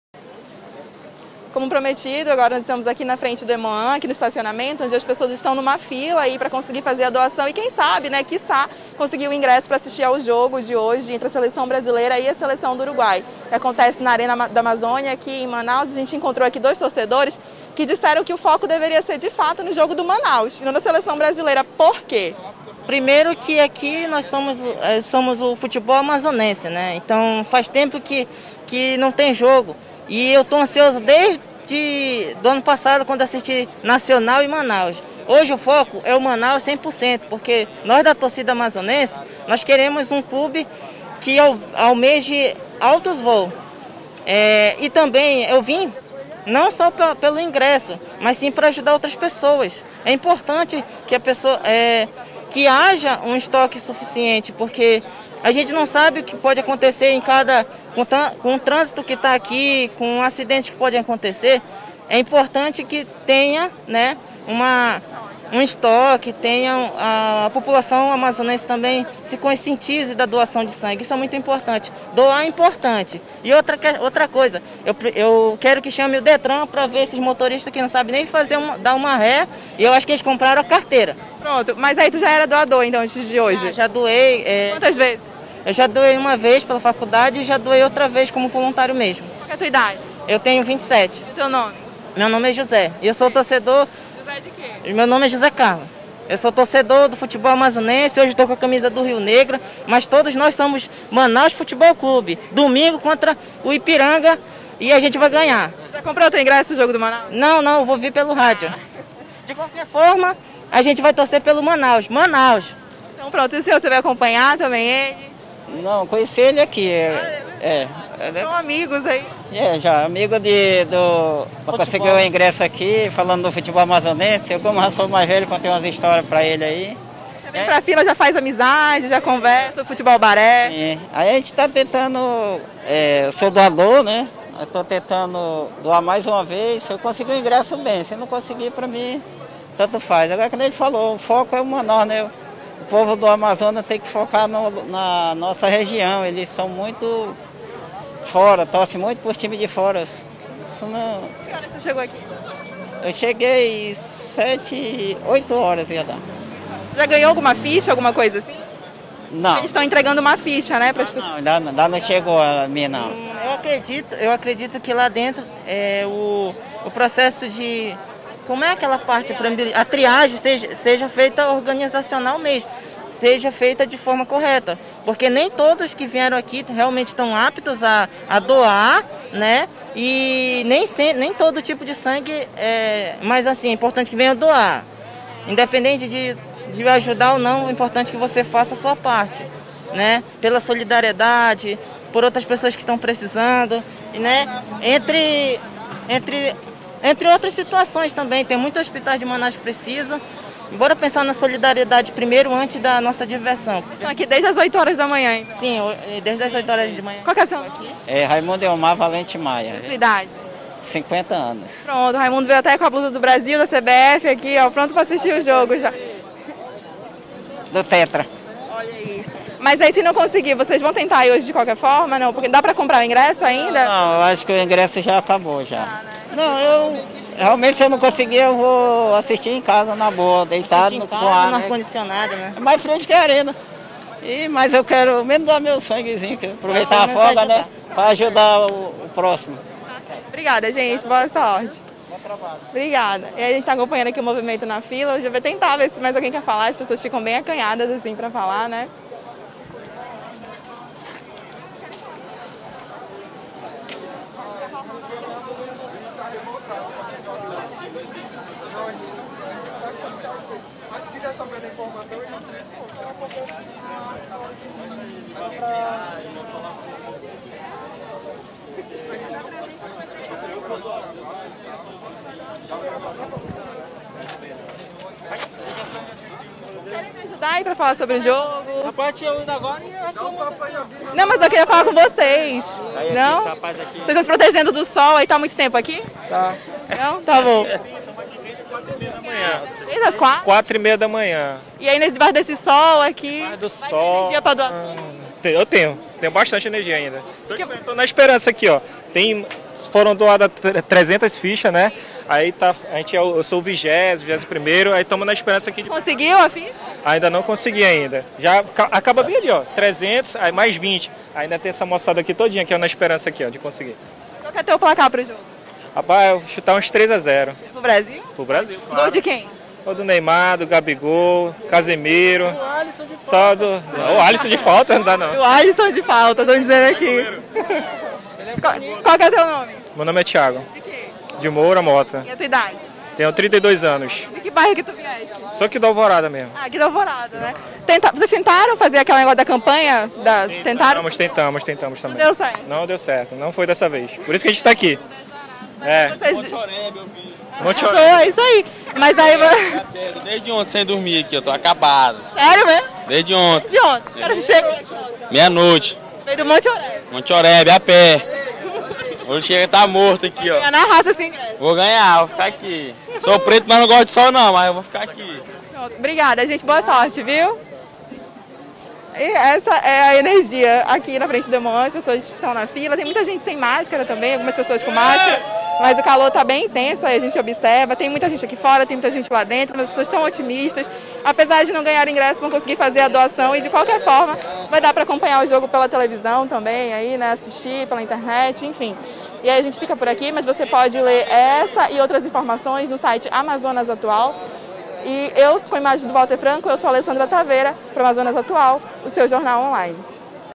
Torcedores amanhecem na fila para obter ingressos e pedem bilhetes também para jogo do Manaus
Dos 3 mil ingressos sorteados para a partida Brasil x Uruguai, apenas 2 mil foram retirados pelos ganhadores na quarta-feira.